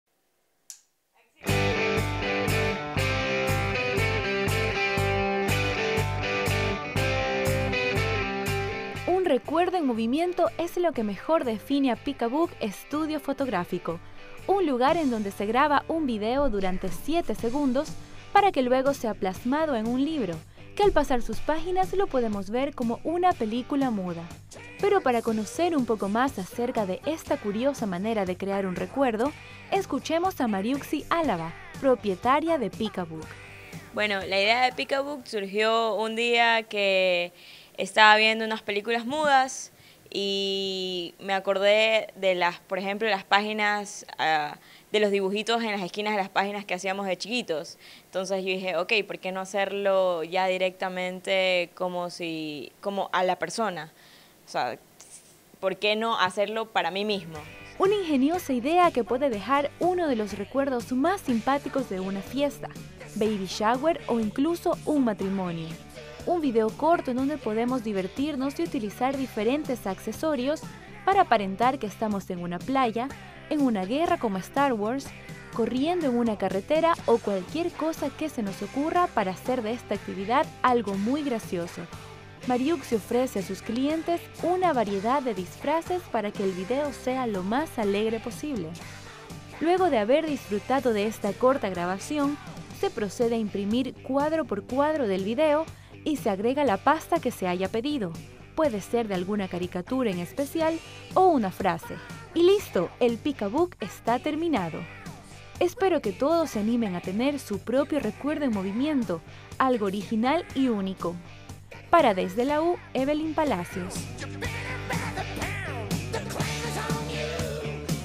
Peekabook Guayaquil
Un recuerdo en movimiento es lo que mejor define a peekabook estudios fotográficos. En esta nota te contamos más.

Desde la U de lunes a viernes a las 16h00